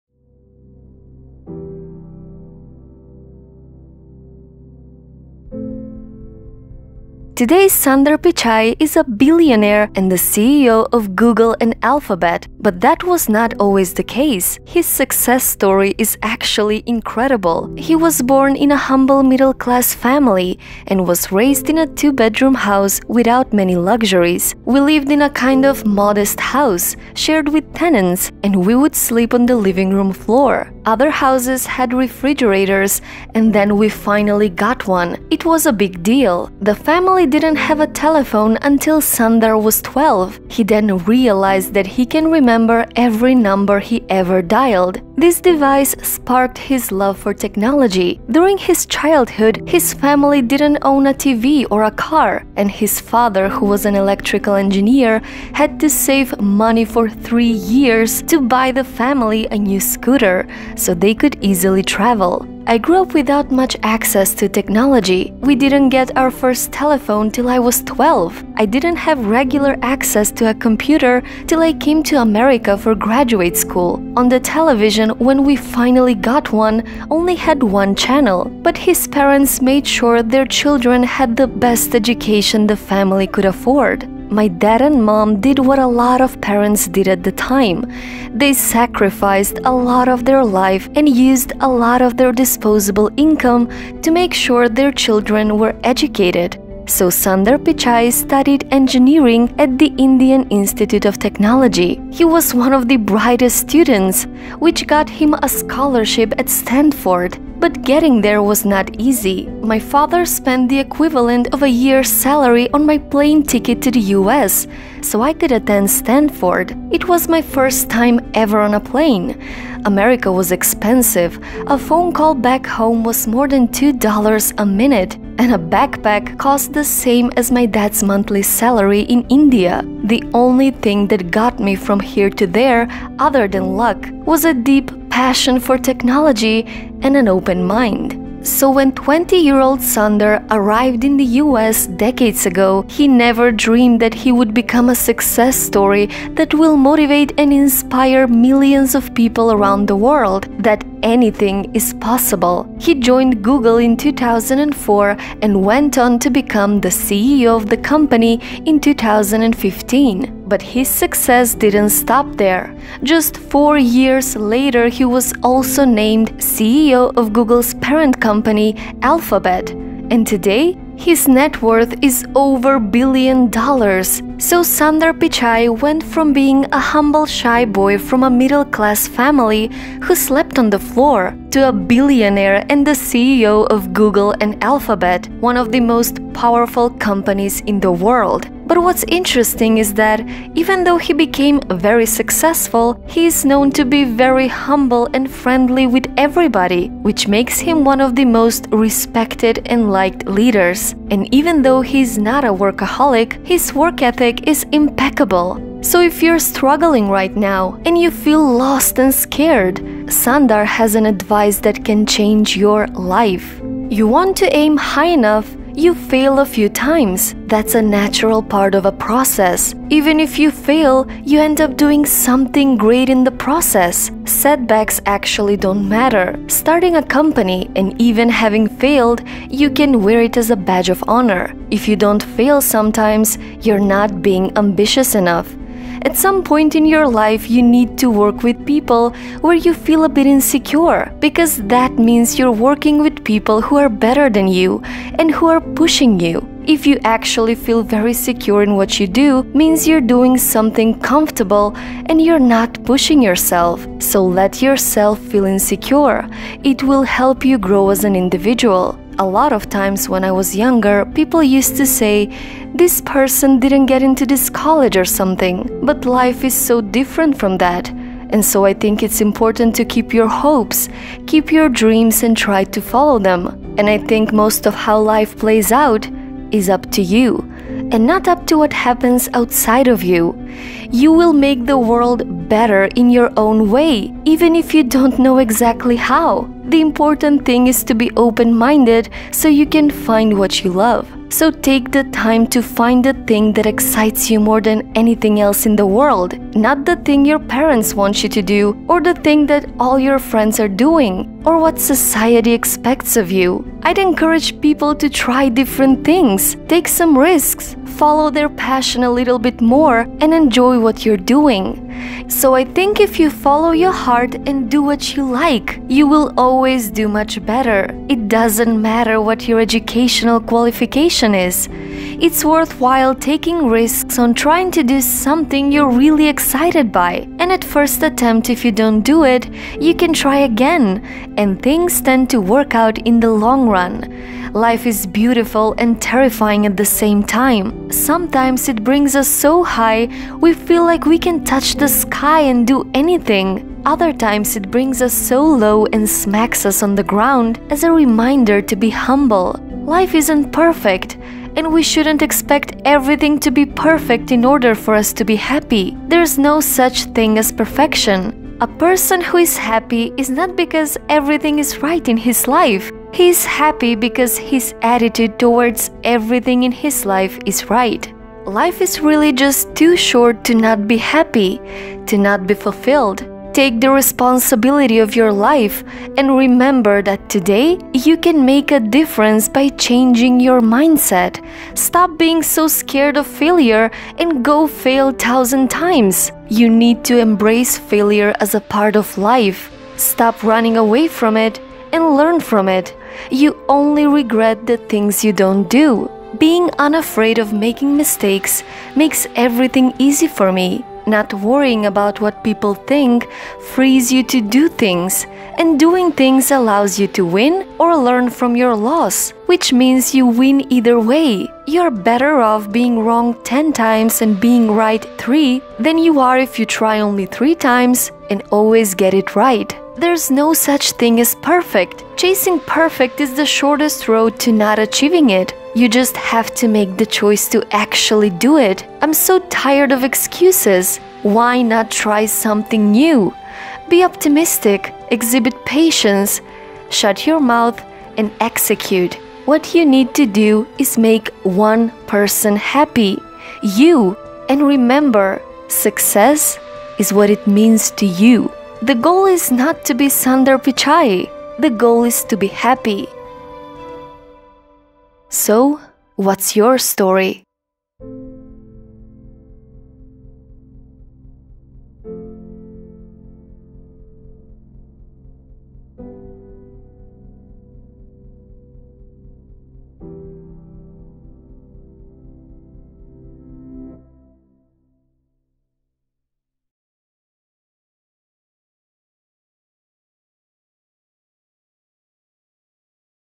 Motivational Success Story Of Sundar Pichai - From Humble Shy Boy To CEO Of GOOGLE and Alphabet
Today, Sundar Pichai is a billionaire and the CEO of Google and Alphabet, but that was not always the case. His success story is actually incredible.
He was born in a humble middle-class family and was raised in a two-bedroom house without many luxuries.

We lived in a kind of modest house, shared with tenants. We would sleep on the living room floor. Other houses had refrigerators, and then we finally got one. It was a big deal."

The family didn't have a telephone until Sundar was 12. He then realized that he can remember every number he ever dialed. This device sparked his love for technology.  During his childhood, his family didn't own a TV  or a car. His father, who was an electrical engineer, had to save money for 3 years to buy the family a new scooter so they could easily travel.

When 20-year-old Sundar arrived in the US decades ago , he never dreamed that he would become a success story that will motivate and inspire millions of people around the world that anything is possible.
He joined Google in 2004 and went on to become the CEO of the company in 2015. But his success didn't stop there. Just 4 years later, He was also named  CEO of Google's parent company - Alphabet.